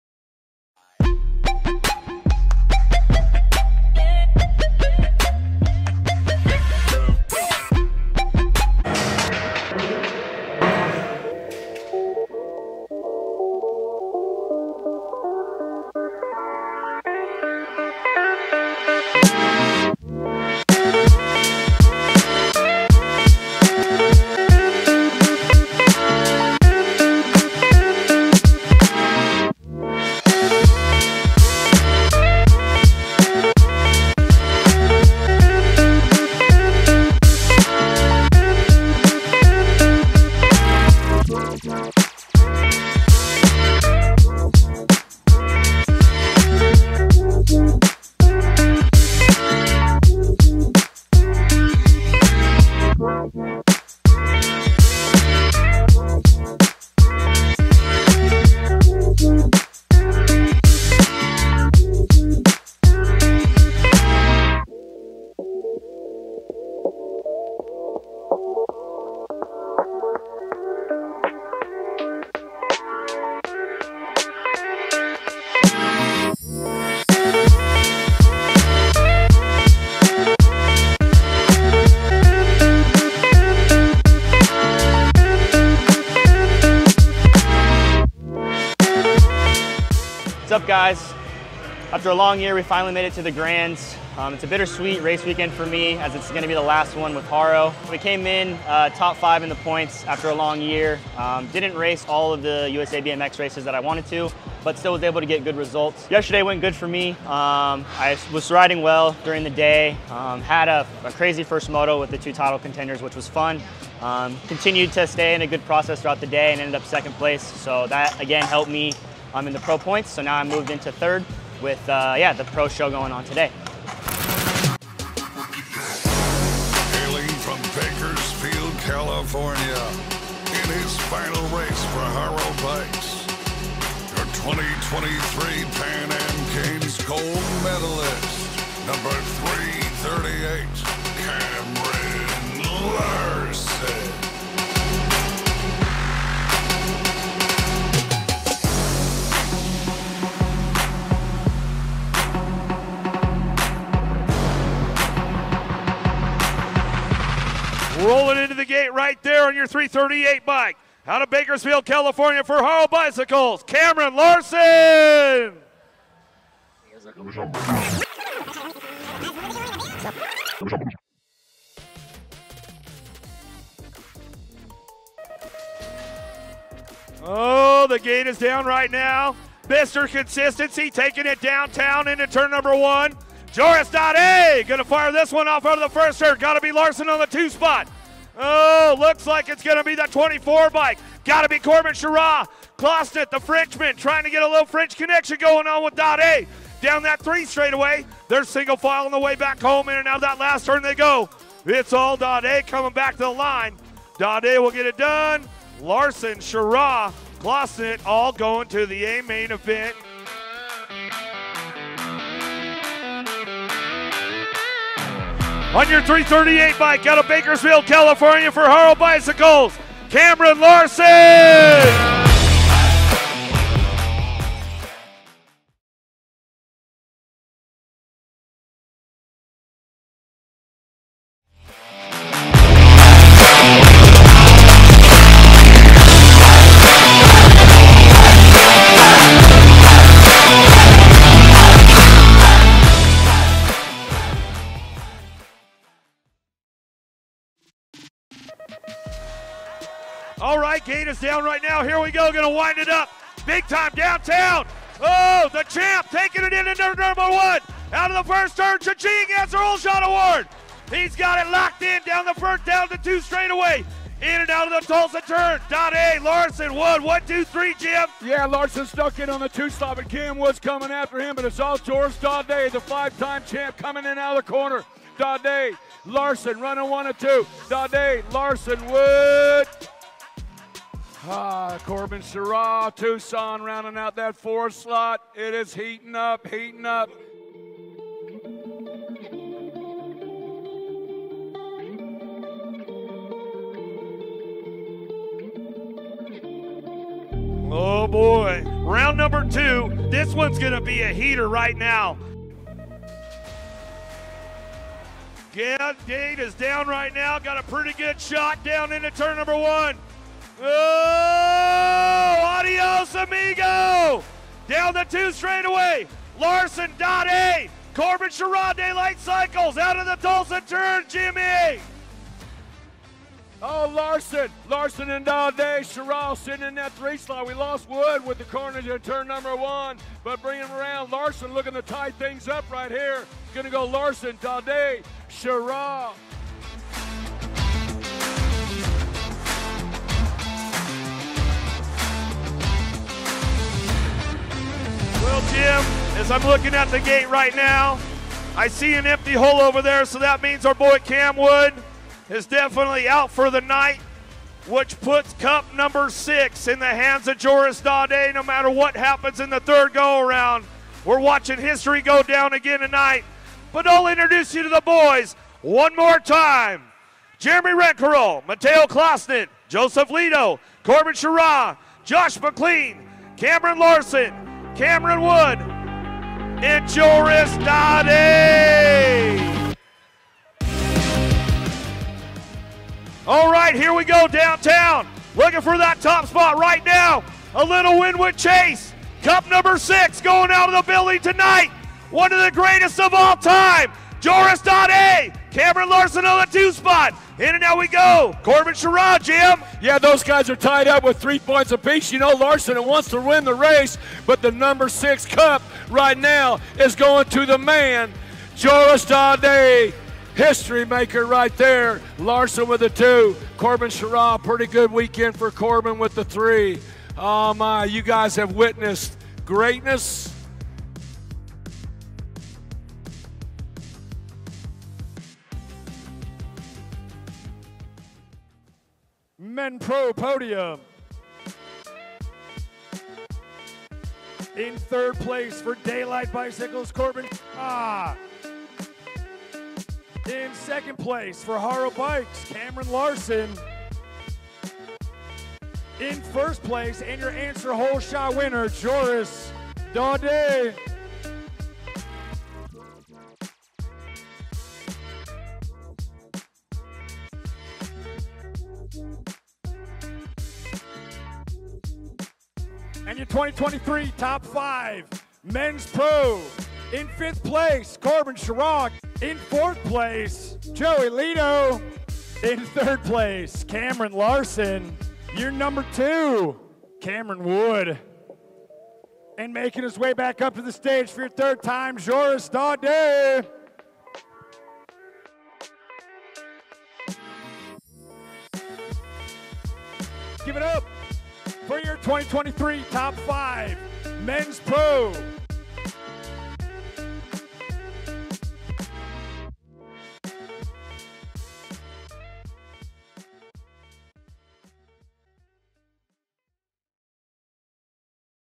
My Last Ride with Haro | 2023 USA BMX Grands
Take a look into my race weekend at USA BMX Grand Nationals. A huge thank you to Stephen Smith for following me around with the camera and editing the video. I am going to do my best to produce more race weekend related content in 2024!